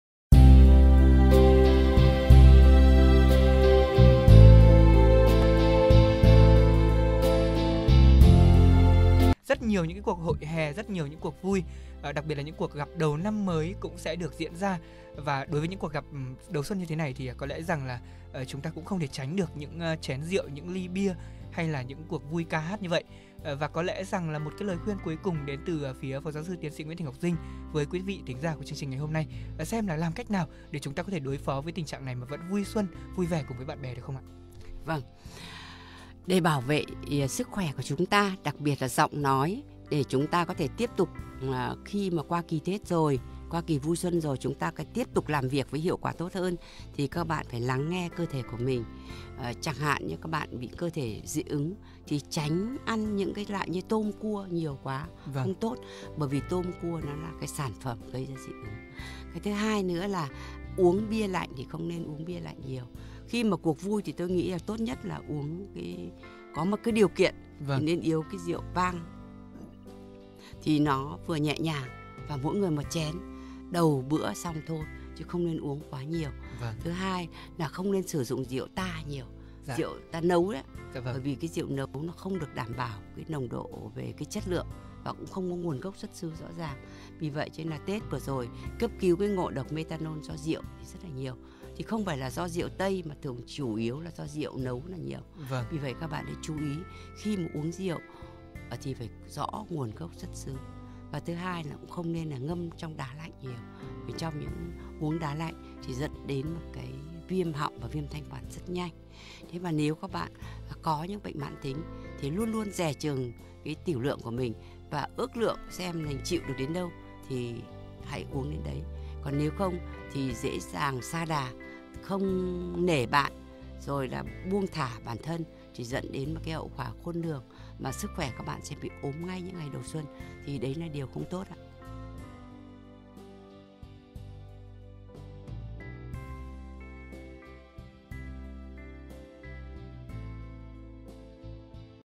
Phòng ngừa khàn tiếng như thế nào hiệu quả? PGS.TS Nguyễn Thị Ngọc Dinh tư vấn
Phòng ngừa khàn tiếng như thế nào hiệu quả? Là mối quan tâm của rất nhiều người. Khàn tiếng, mất giọng là tình trạng phổ biến trong cuộc sống hàng ngày, không chỉ khiến cho những ai mắc phải luôn gặp khó khăn trong giao tiếp mà còn gây ảnh hưởng lớn đến chất lượng công việc cũng như các hoạt động thường ngày.
Vậy, phải làm sao để phòng ngừa khàn tiếng? Hãy cùng lắng nghe PGS.TS Nguyễn Thị Ngọc Dinh tư vấn cụ thể trong nội dung video trên đây.
-Nếu bạn có bất kỳ băn khoăn nào về tình trạng khản tiếng, mất tiếng hay các bệnh viêm đường hô hấp trên bạn có thể gọi tới tổng đài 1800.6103 (MIỄN CƯỚC CUỘC GỌI)/ kết bạn Zalo/ Viber: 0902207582  để được tư vấn tối đa.